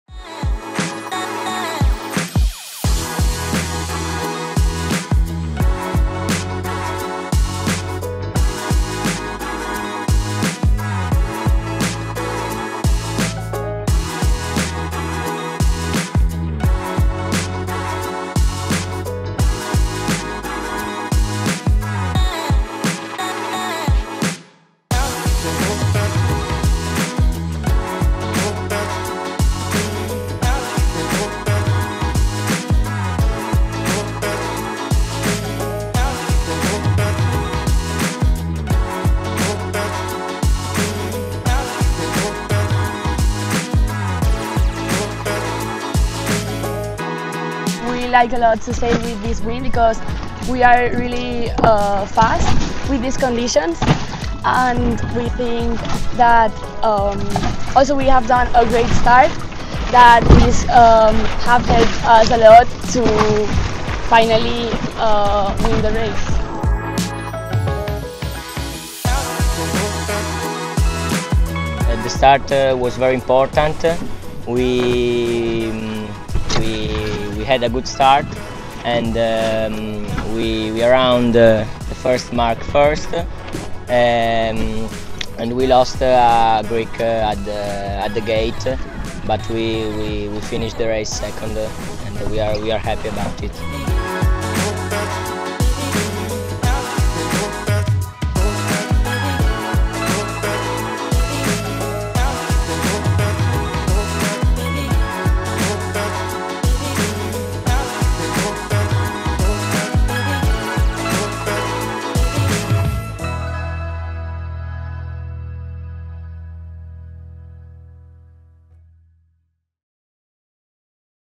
420 Open European Championship Portoroz Day 5
The 2024 420 Open European Championships is taking place from July 1st to July 9 on Portoroz - Slovenia.

243 crews from 22 different countries are competing in the Gulf of Trieste for the Open European Champion Title.